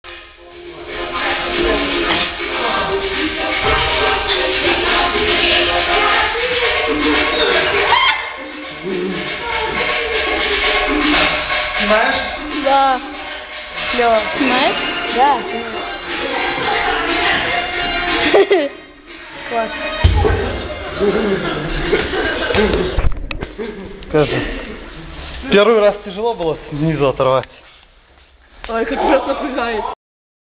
"Жим живой сотни =)"
2 ребенка по 42кг + гриф 20кг=104кг